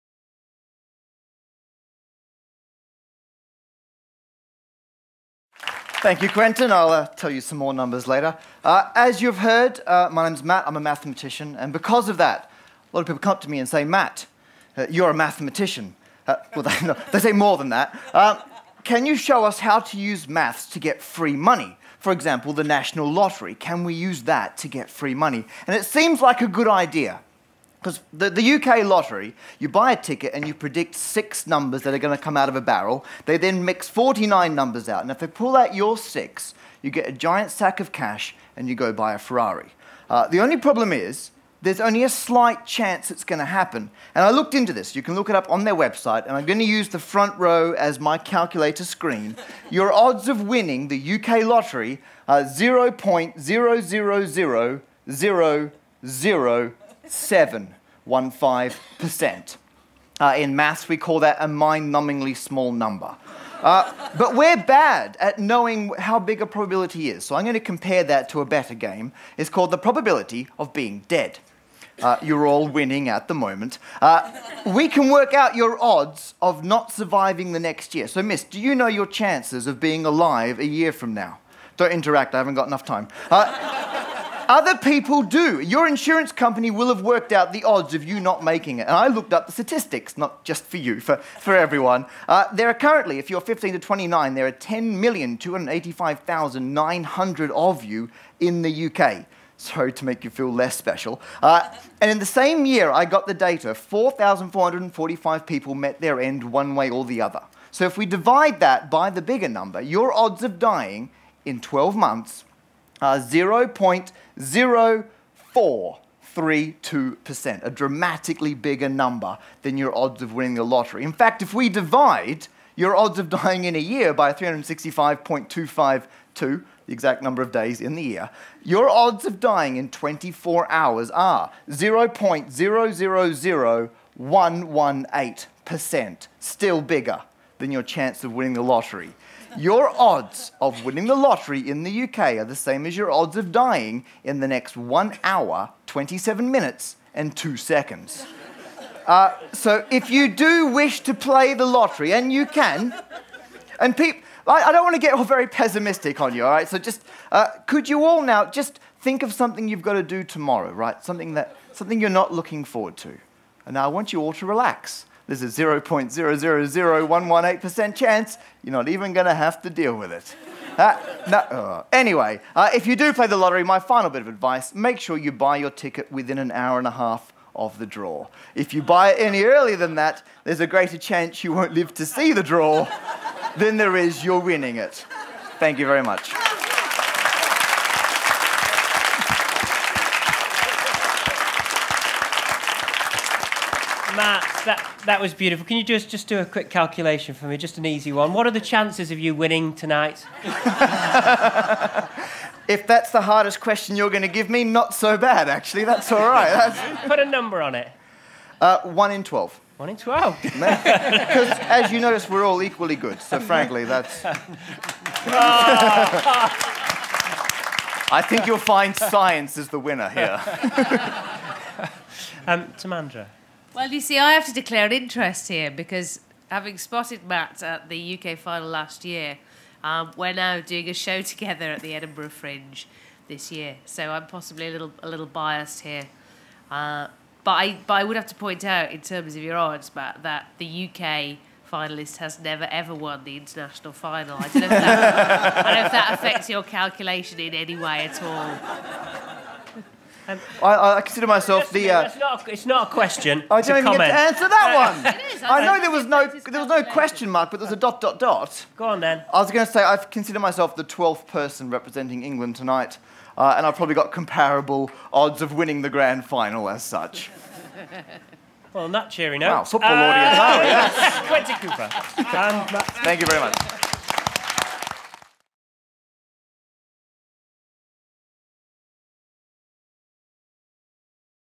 Matt Parker (UK) - International Final 2010
Matt Parker - the UK entrant for Famelab International 2010 held during The Times Cheltenham Science Festival